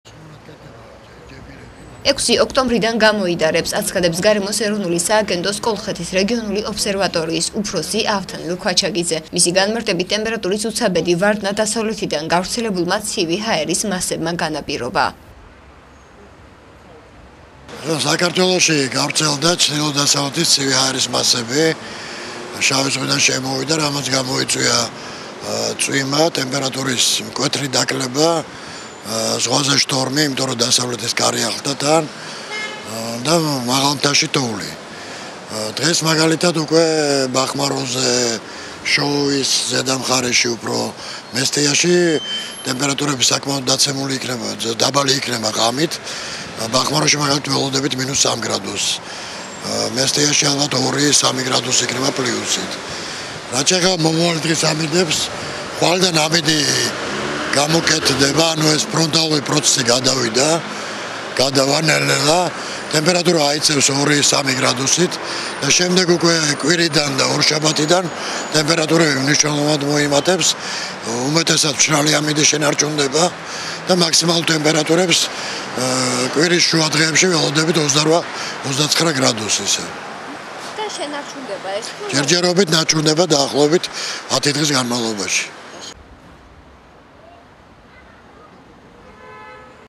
6 ოქტომბრიდან საქართველოში გამოიდარებს
6 ოქტომბრიდან საქართველოში გამოიდარებს.  აცხადებს  გარემოს ეროვნული სააგენტოს კოლხეთის რეგიონალური ობსერვატორიის  უფროსი ავთანდილ ქვაჩაკიძე.   მისი განმარტებით ტემპერატურის უცაბედი ვარდნა  დასავლეთისდან  გავცელებულმა ცივი ჰაერის მასებმა გამოიწვია.